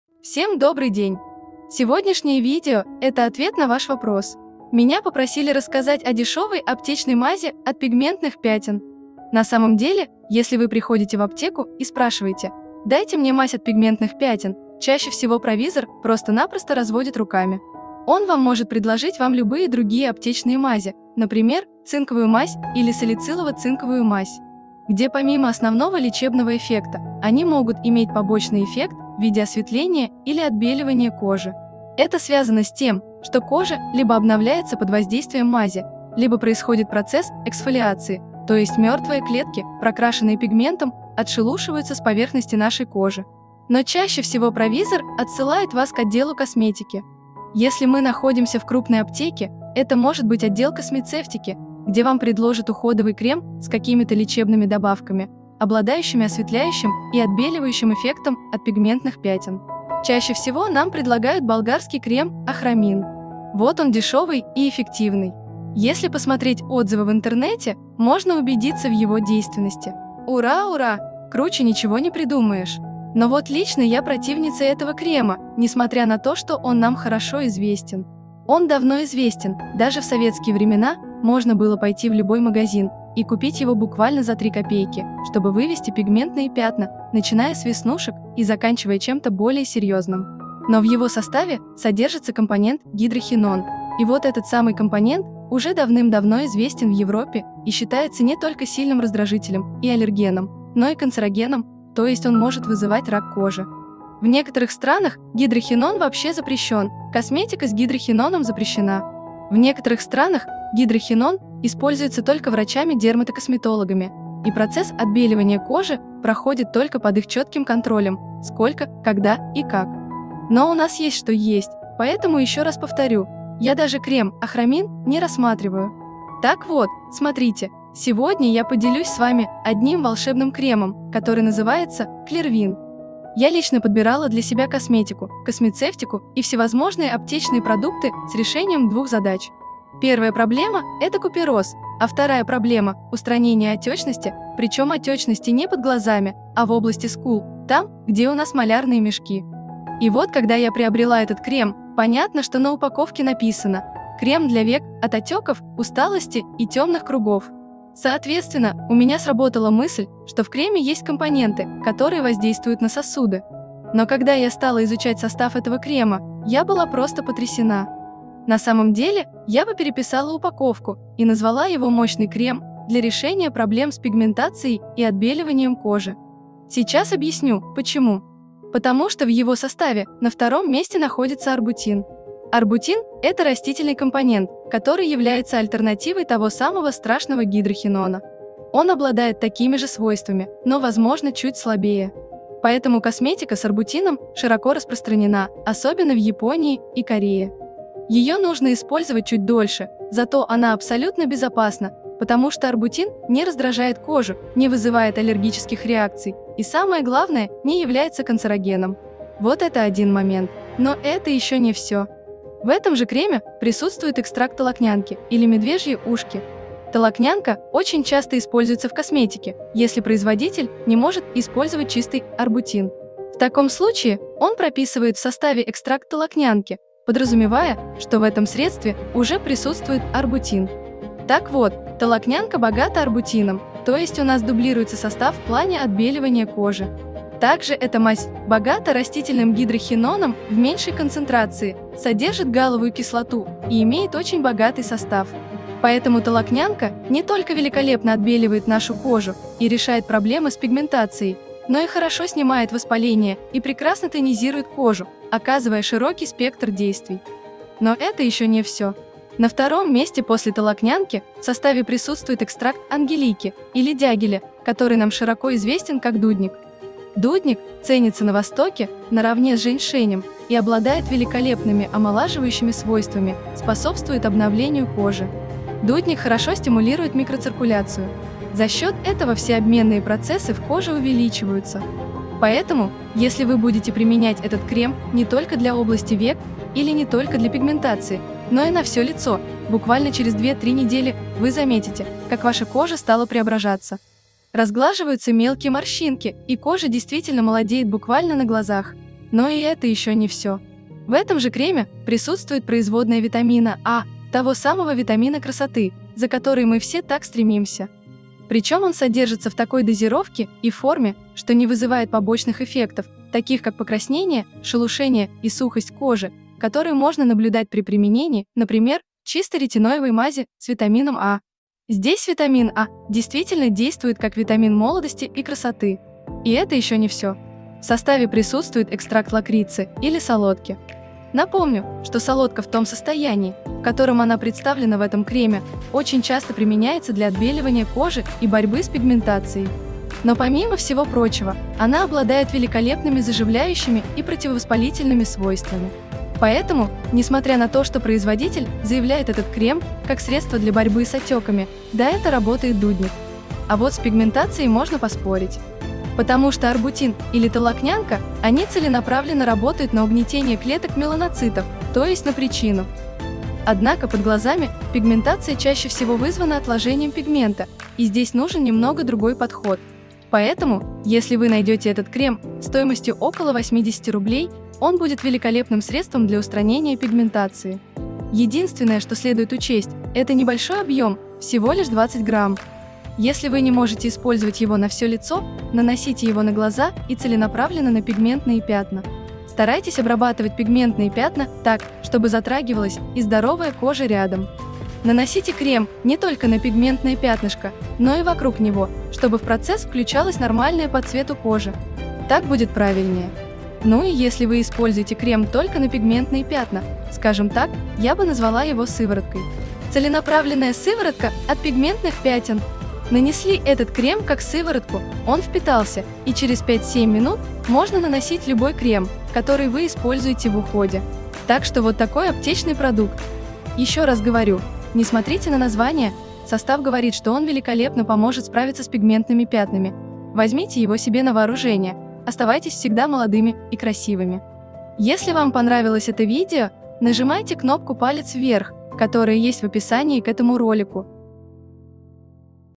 ЛУЧШИЙ крем ОТ ПИГМЕНТНЫХ ПЯТЕН и ОТБЕЛИВАНИЯ кожи. Пигментные пятна на лице как избавиться
Пигментные пятна на лице и как от них избавиться с помощью крема, чтобы отбелить лицо. Пигментные пятна не являются заболеванием кожи, однако они представляют собой серьезный косметический дефект. Основная причина появления пигментных пятен — повышенный уровень меланина в клетках кожи. Существует множество факторов, провоцирующих образование на теле пигментных пятен.